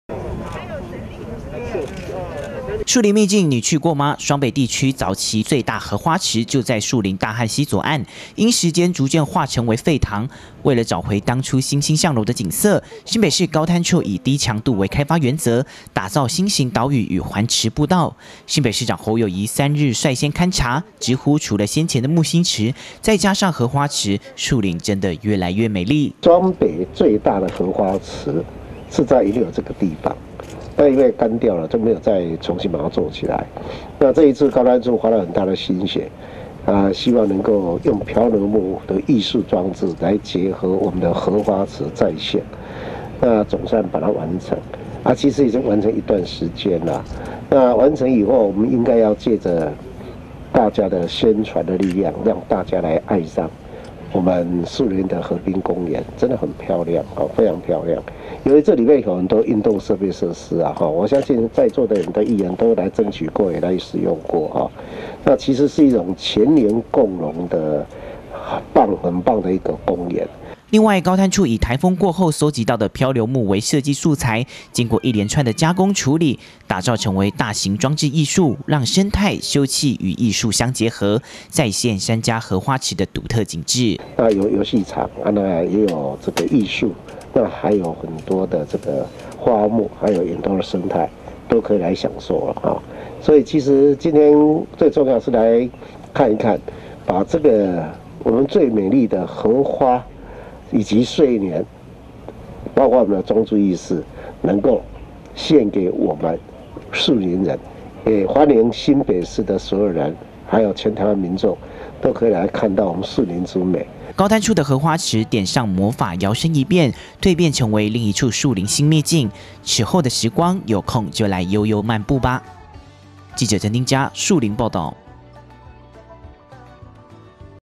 荷花 秘境│樹林再出秘境 漂流木「荷」你遨遊
位在大漢溪左岸的樹林山佳荷花池，在高灘處的改造之下變成樹林新秘境，新北市長侯友宜3日先行探訪並表示，河濱公園一直以來都是市民休閒娛樂的去處，繼先前的沐心池，這次重新整頓荷花池，期望能提供一個更好、更優質的河濱休憩環境。